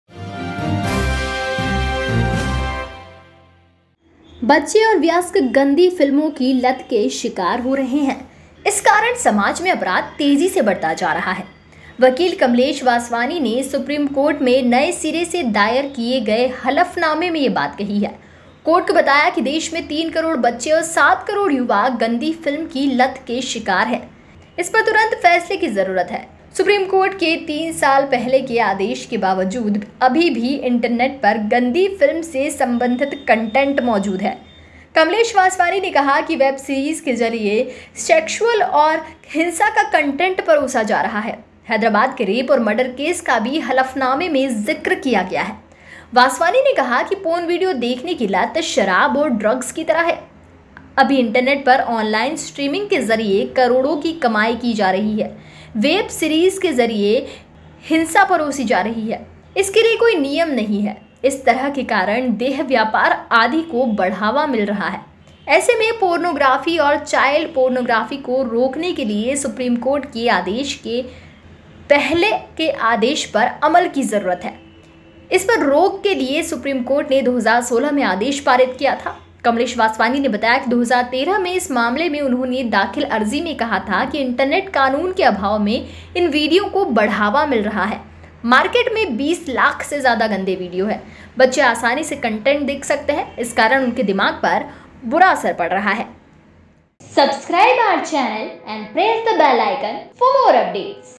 क्या गंदी फिल्मों के कारण हिंसक हो रहे हैं बच्चे और युवा
गंदी फिल्में बर्बाद कर रही है